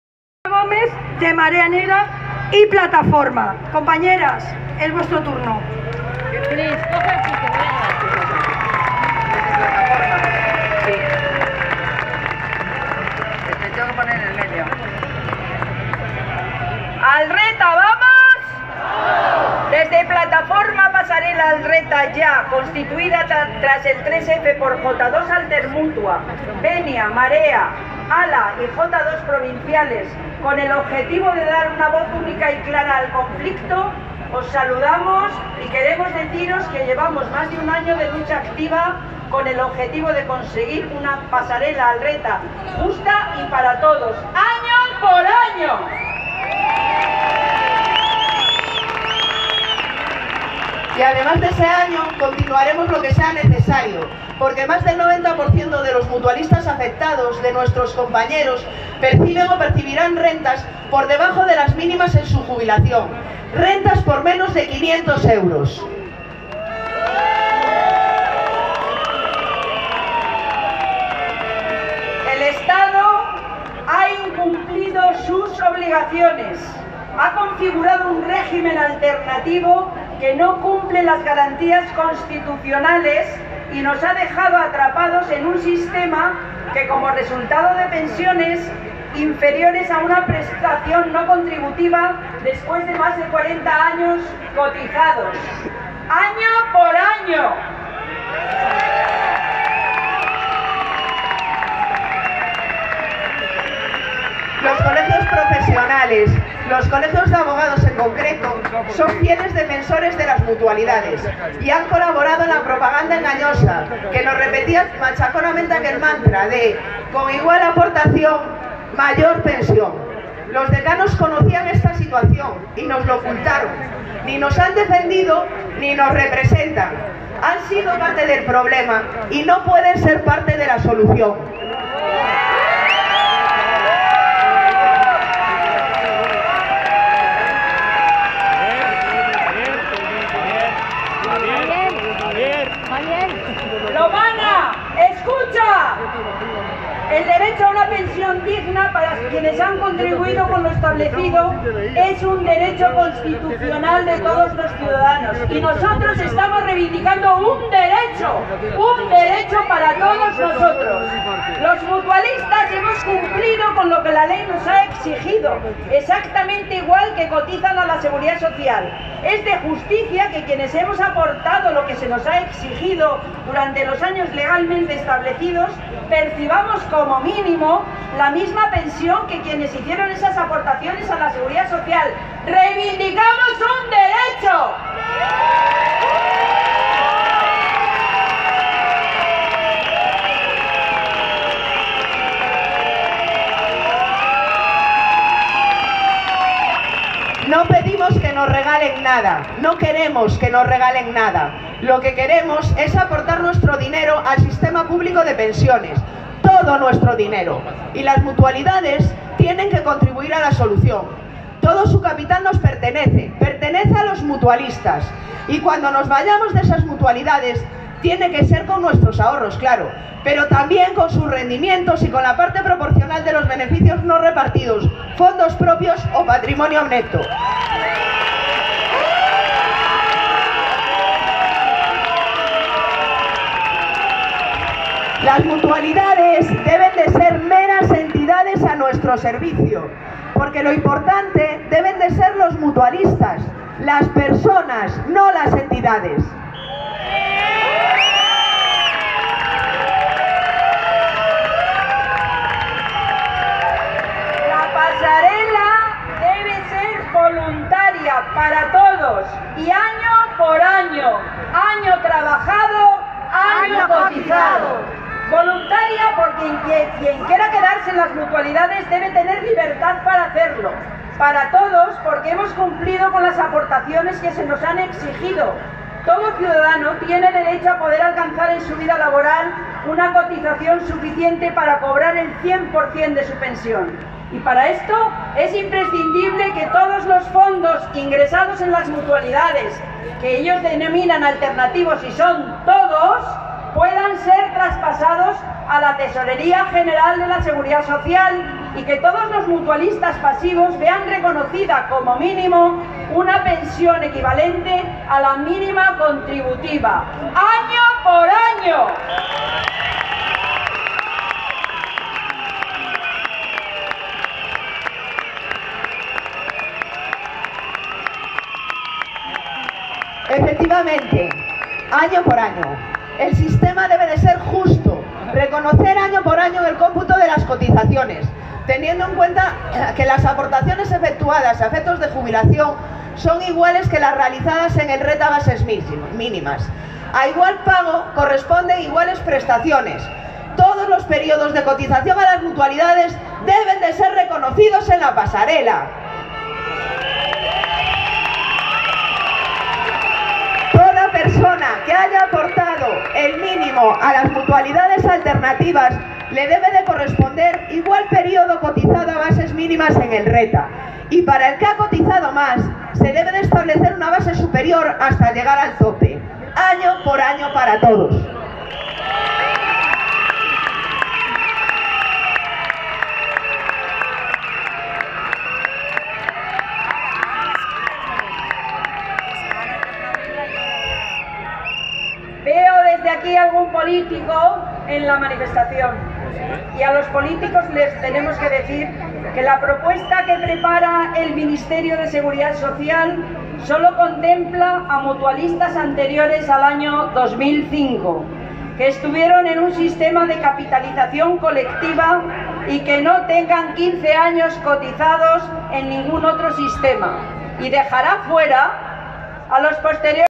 Abogados y procuradores del Bierzo reivindican en Madrid "unas pensiones dignas"
Más de 10.000 abogados de todo el país han acudido este sábado, 28 de septiembre, a Madrid para exigir que cada año que hayan aportado a la Mutualidad pase automáticamente a ser cotizado al Régimen Especial de Trabajadores Autónomos (RETA).